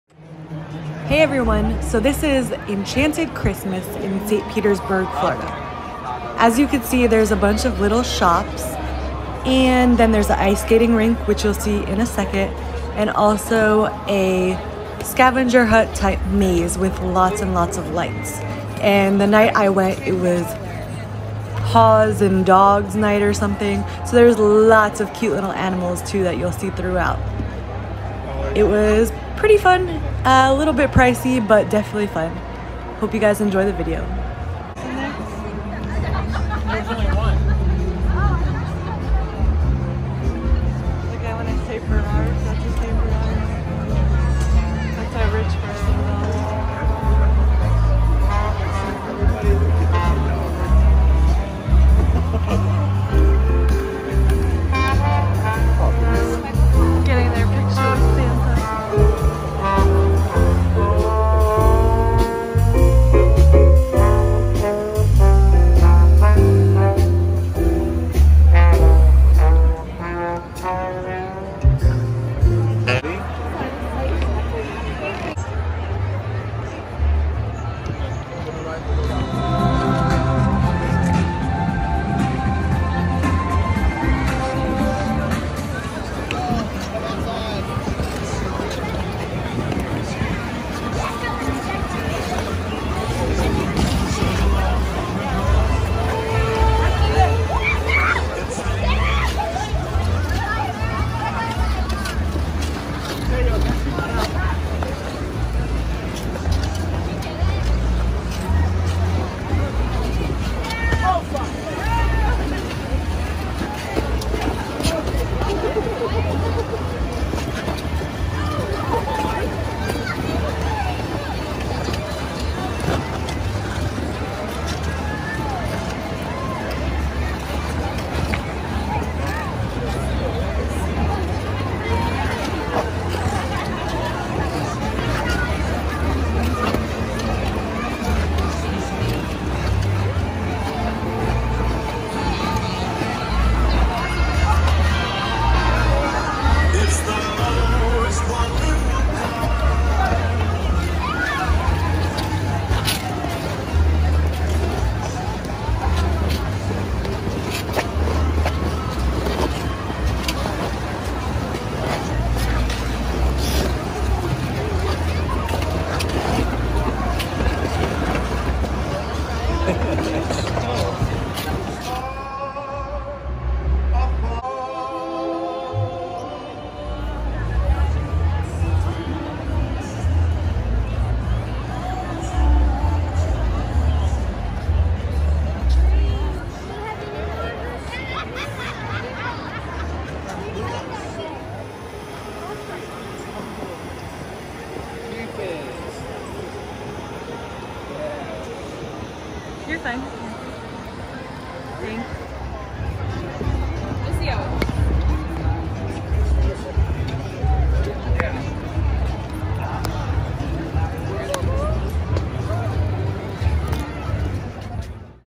Enchanted Christmas-St. Petersburg, Florida
I visited Enchant Christmas in St. Petersburg, Florida. It was a magical night of Christmas light Mazes, Ice skating and hot cocoa!